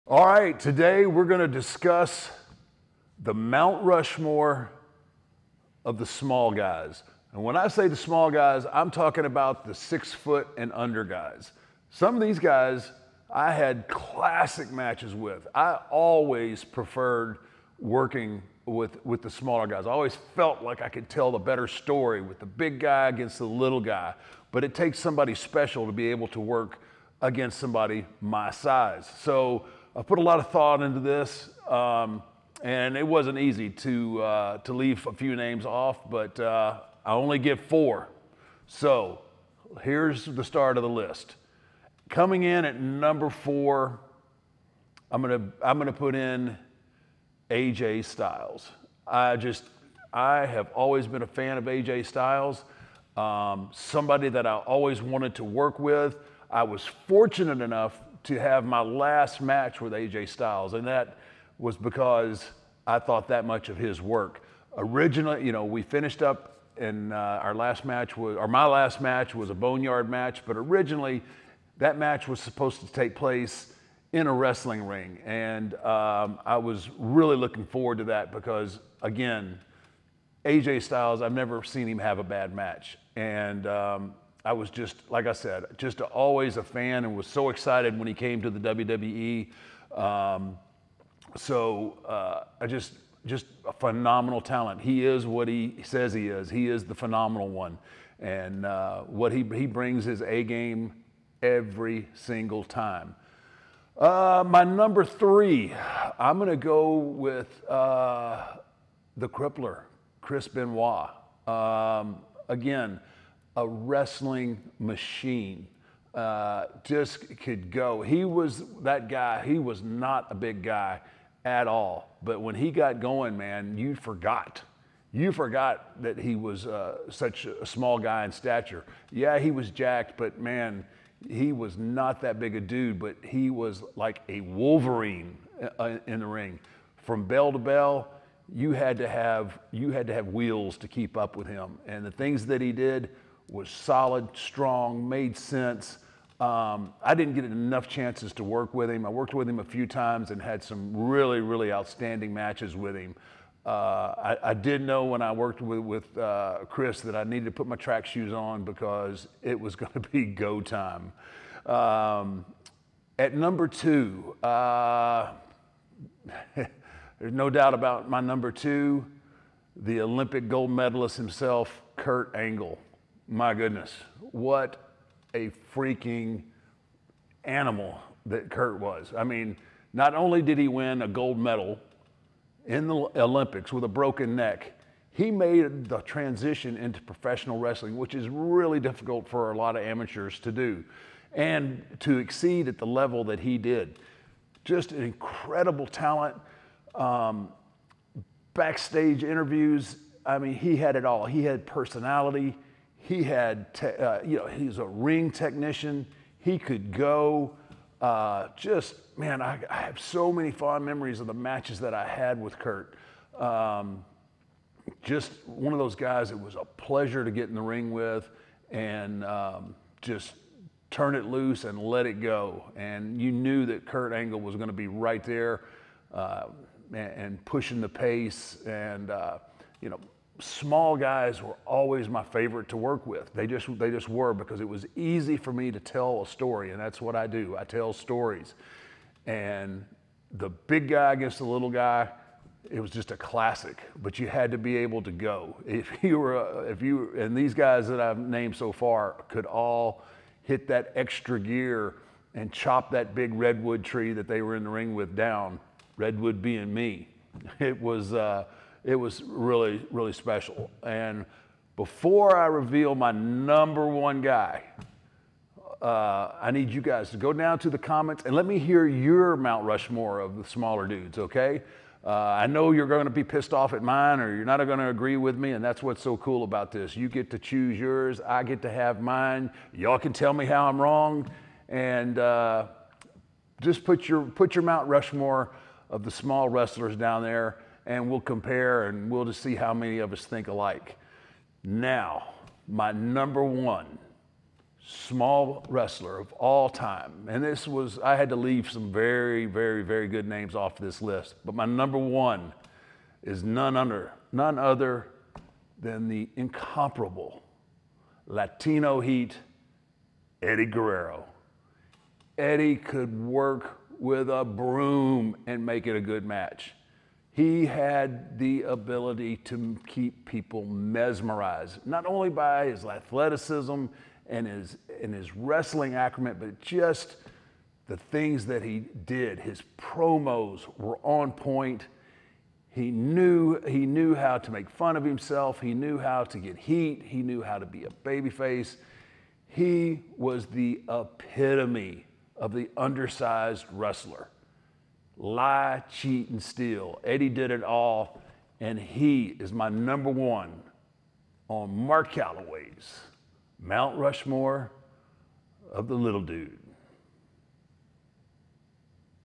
Undertaker Rates His Top WWE Small Guys of All Time
Mark makes some tough choices for his Mount Rushmore of Wrestling Small guys.